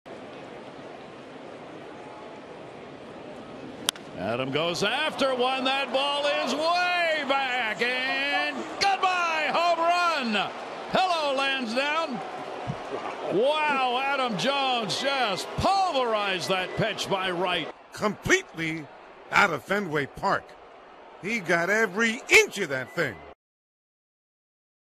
BAL@BOS: Jones lifts a deep homer to left field
Adam Jones lifts a solo home run to deep left field, extending the O's lead to 7-0 in the top of the 2nd inning